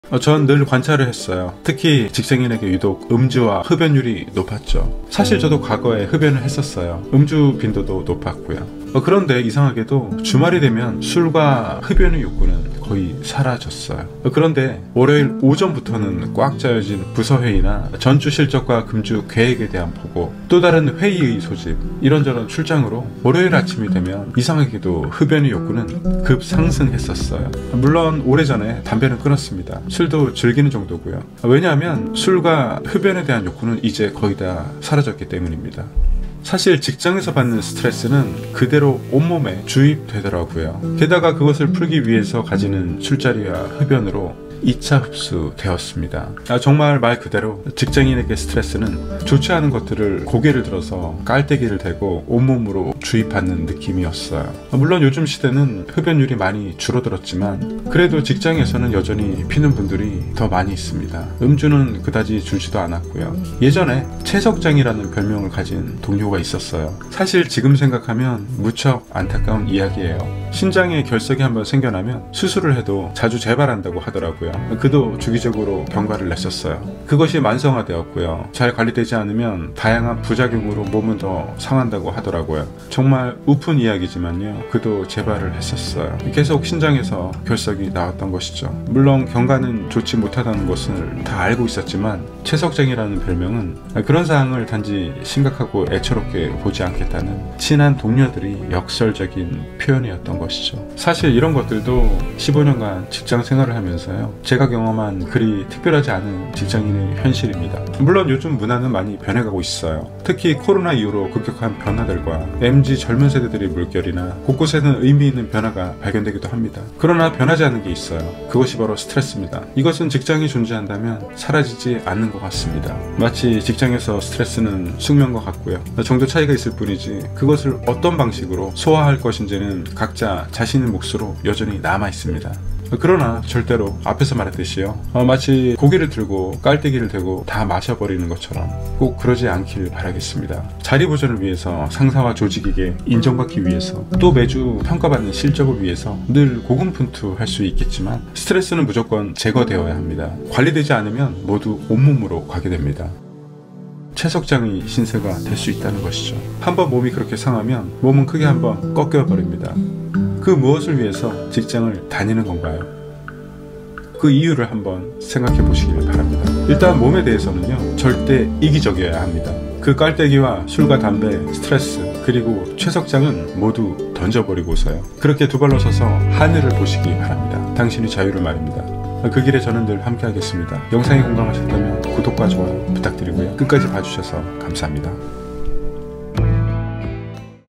직장생활 15년하고 깨달은 진실(솔직함 주의)
직장생활을 15년하고 깨달은 진실 입니다.
솔직한 경험을 남깁니다.

직장을 벗어나는 콘텐츠 사업가의 길 pdf 1편 다운로드
영상은 자막은 vrew를 활용하여 생성했습니다.